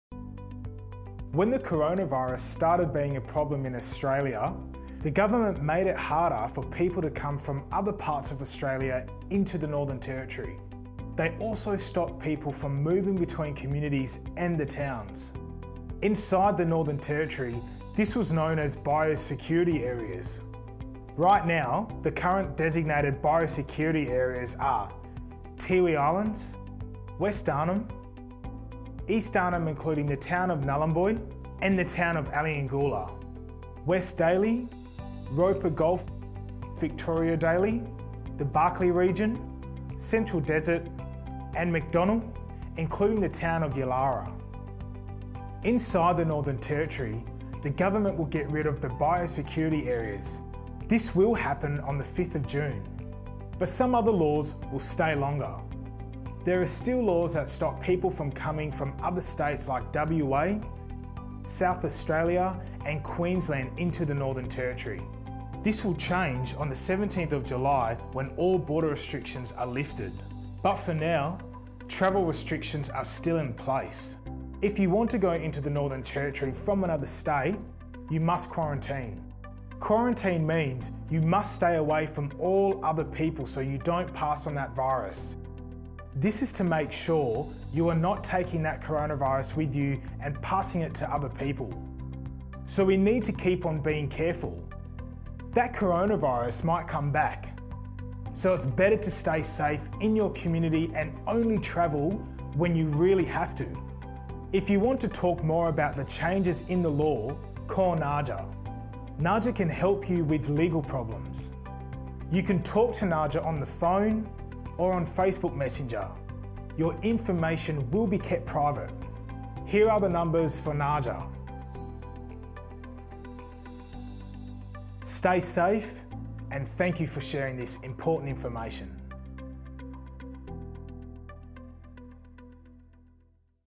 About Bio-Security areas
Community Legal Education (CLE) video to inform the community about Bio-Security areas and laws during COVID-19 period.  Please note the contents of this video is accurate at the time of publication.